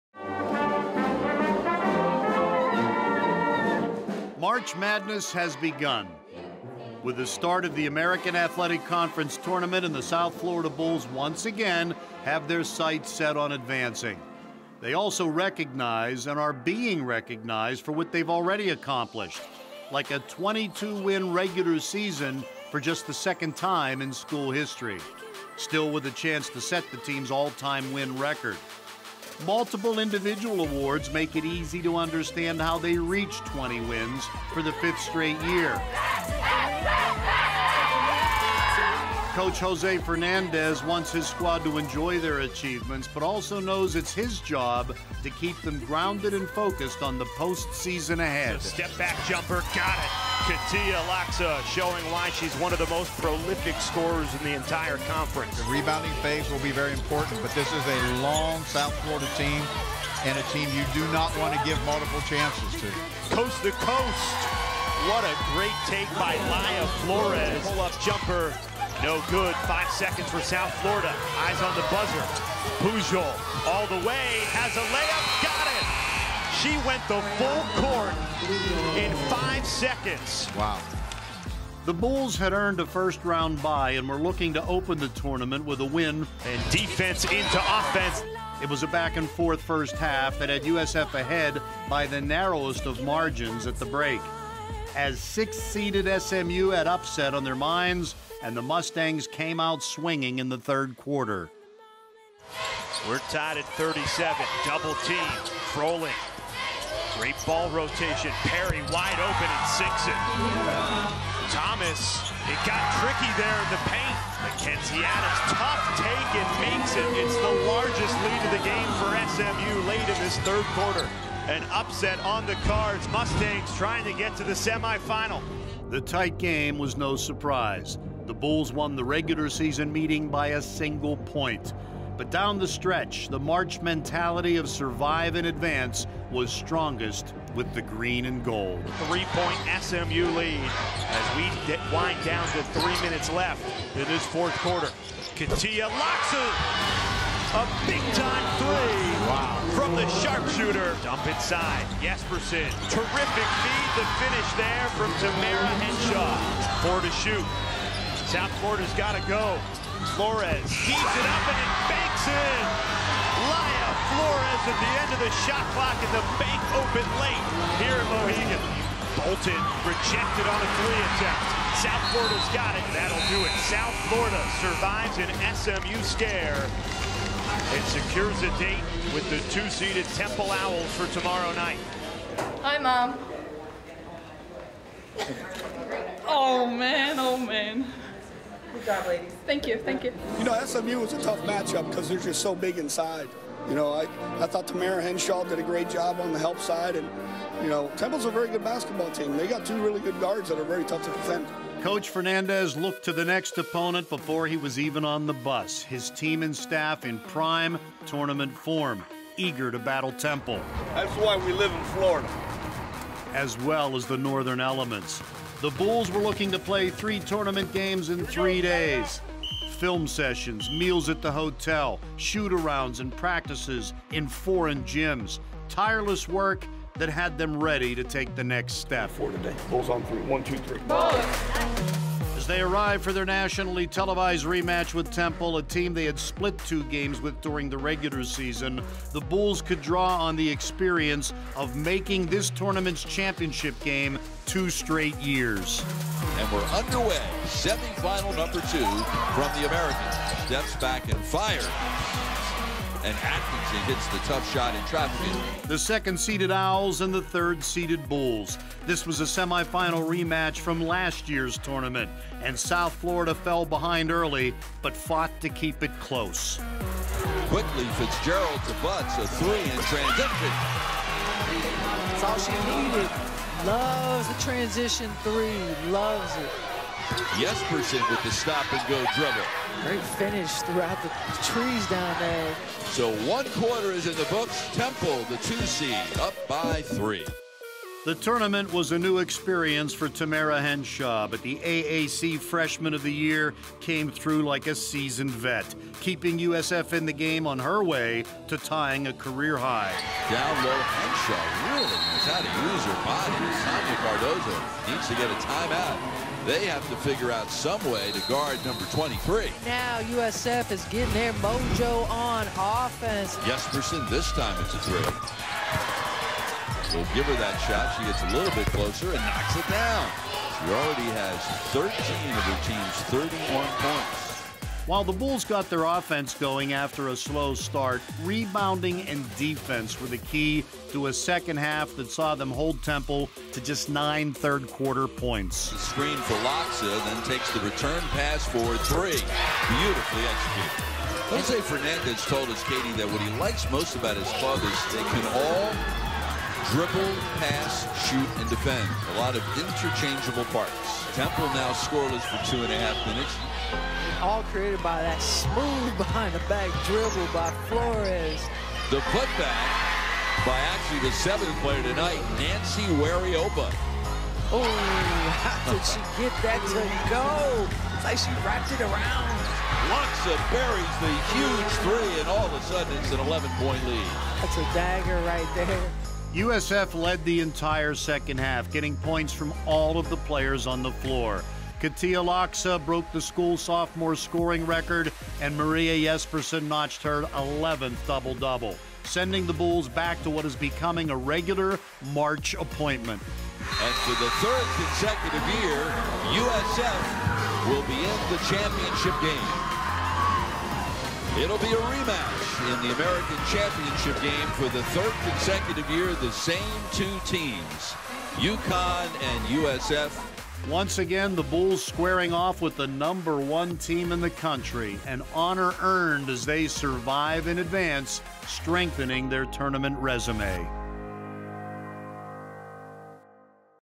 USF Basketball 2017: Conference Tournament
Highlights from the women's run to the finals of the American Conference Tournament!